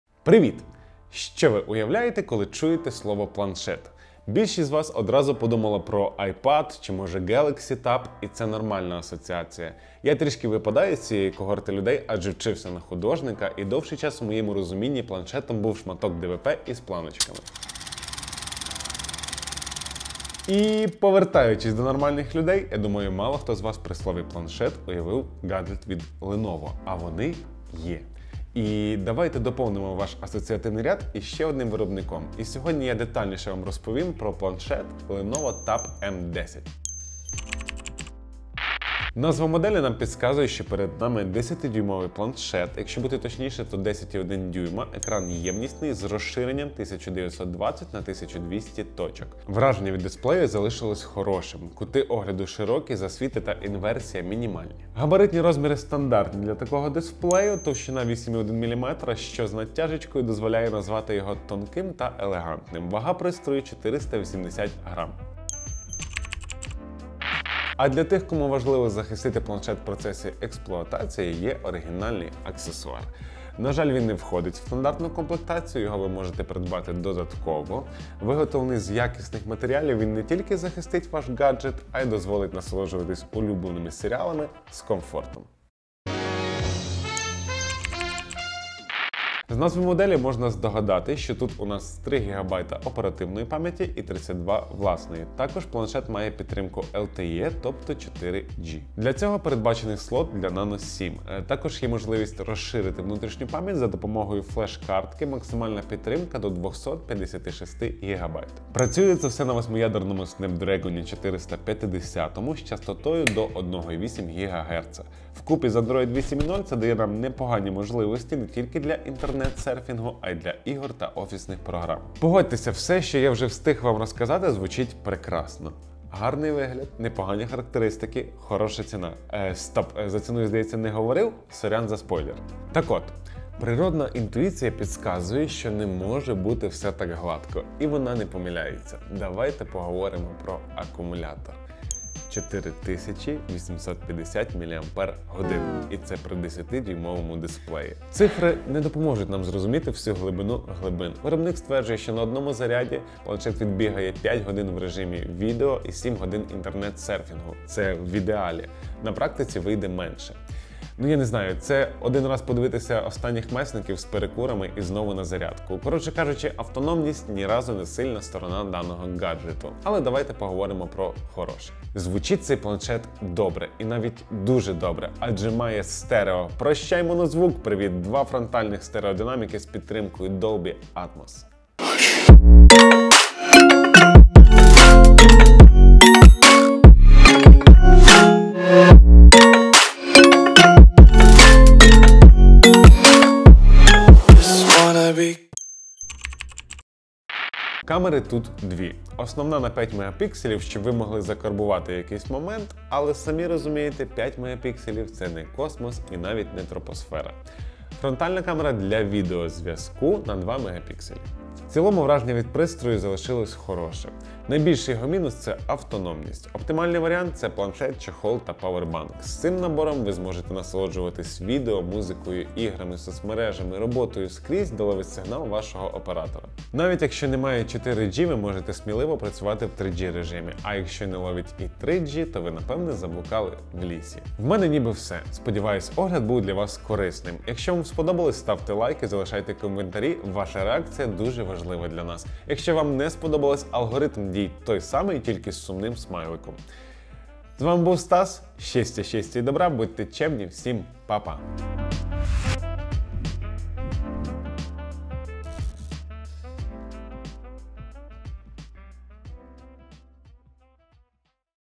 Lenovo Tab M10 TB-X605L. Планшет для дому? Огляд.
Lenovo Tab M10 TB-X605L 10" LTE 3/32GB - планшет середньої цінової категорії з яскравим дисплеєм та непоганим функціоналом. Про плюси та мінуси моделі дивіться у огляді.